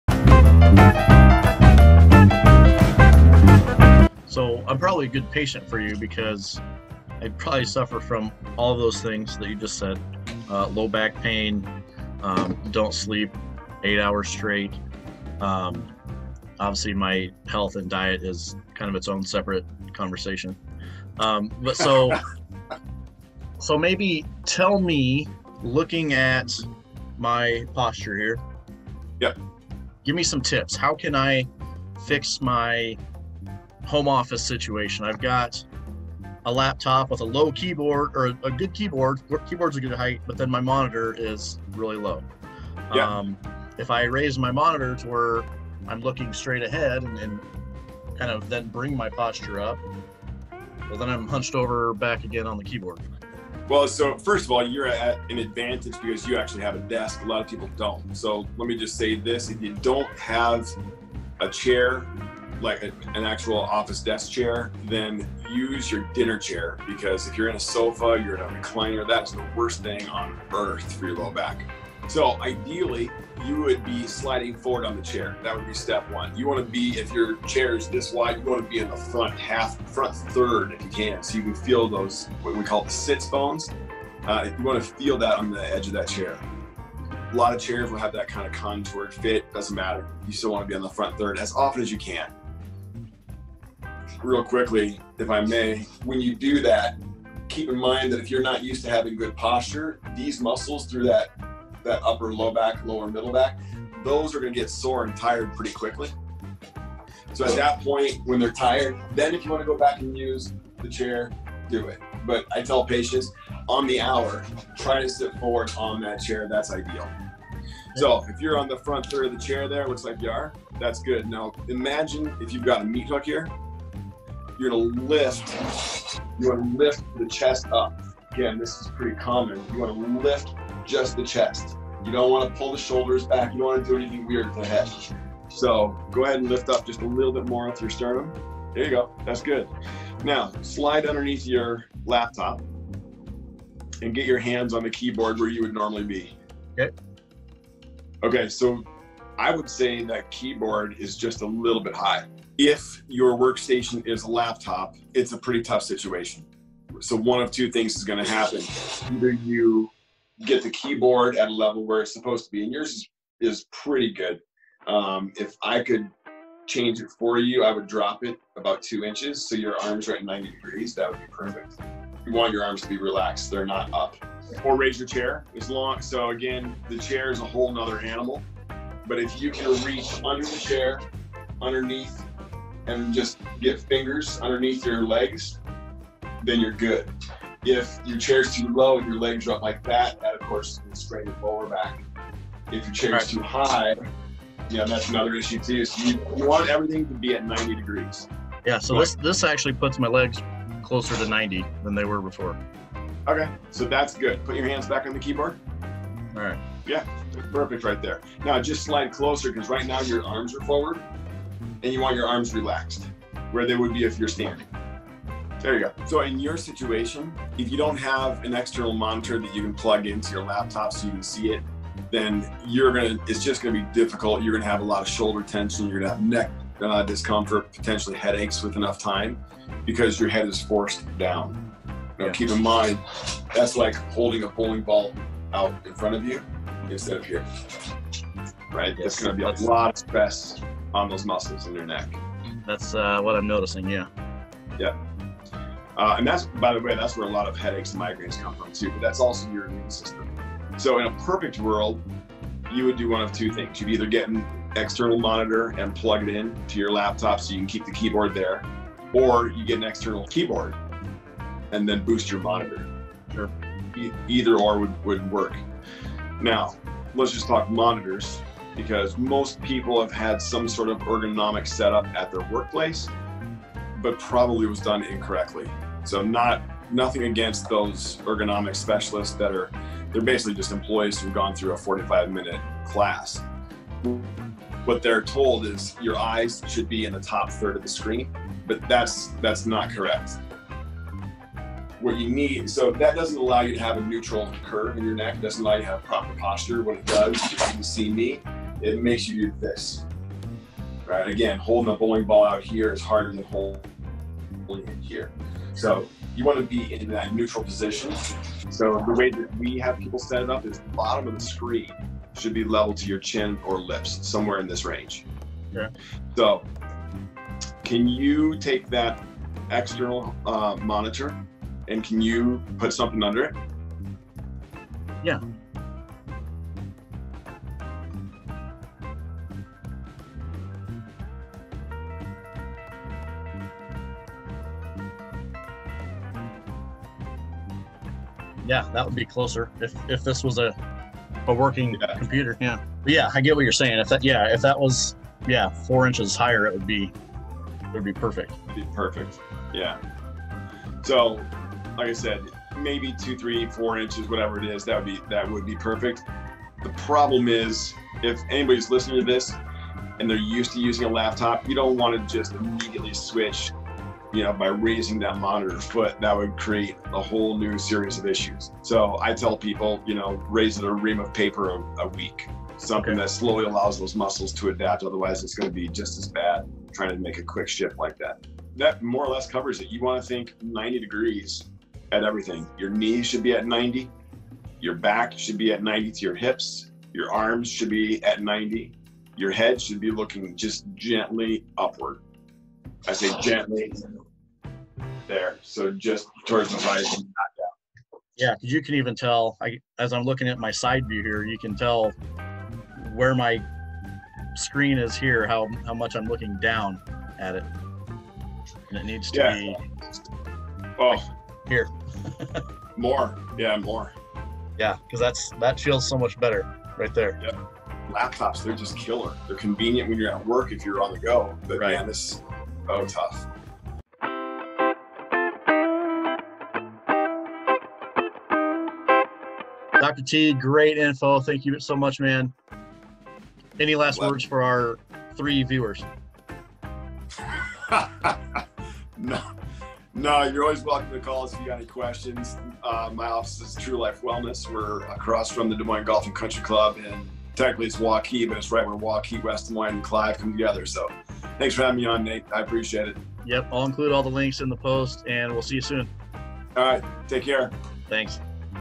Home office posture with Dr. Tyler Molstre of True Life Wellness - Part 2/2
Part 2 of my chat with Tyler Molstre, D.C. of True Life Wellness about posture and how a bad desk setup can contribute to back pain, poor posture, and lowered immunity.

If you're feeling a little more tension in your neck or your back doesn't feel great after a day of working at home, Dr. T gives us some great tips on setting up our home office to help relieve some of that pain.